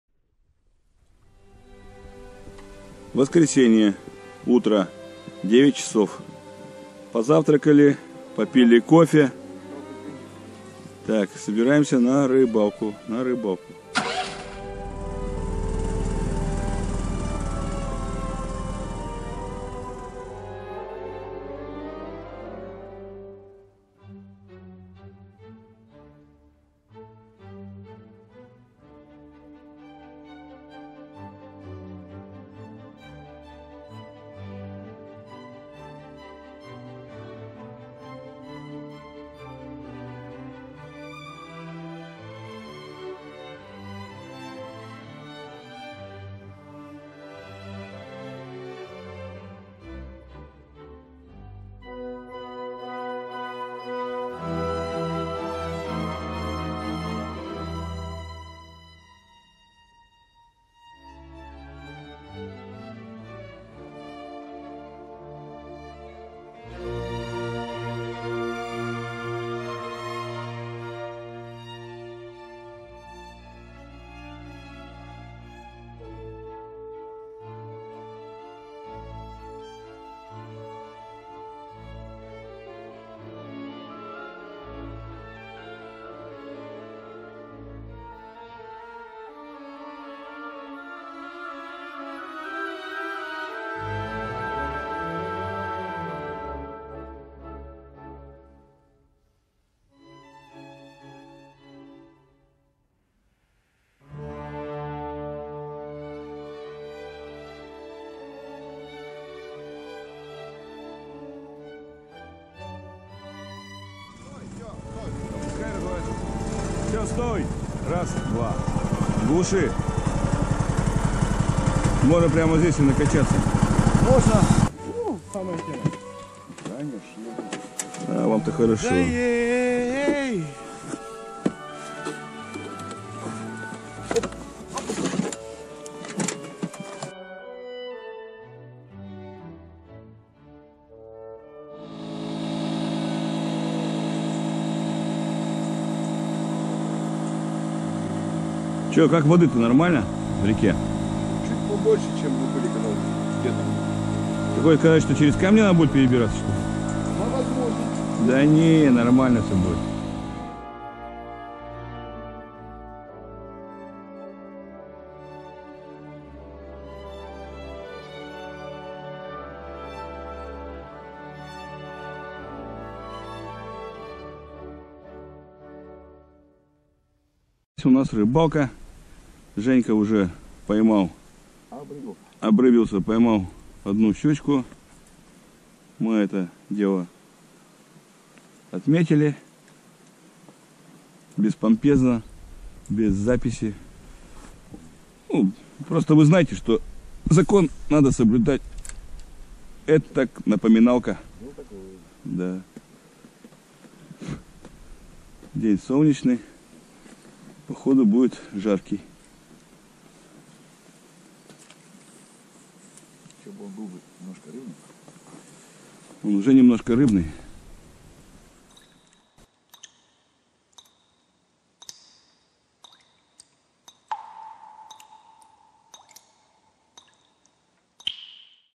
Карповская в сентябре 2023. Катимся к реке, качаем лодки. Начинается рыбалка.
С утра небо стало просветляться. Собрались на реку и свезли лодки на берег. Накачали. Отогнав каракат к дому немного поснимал на смартфон то, что когда то было весёлым селом. Дома, что от них осталось, совсем пропадают, скоро не останется и стен. Так что, это память электронная помогает нам запечатлеть хоть что то. Всем добра! Про рыбалку далее.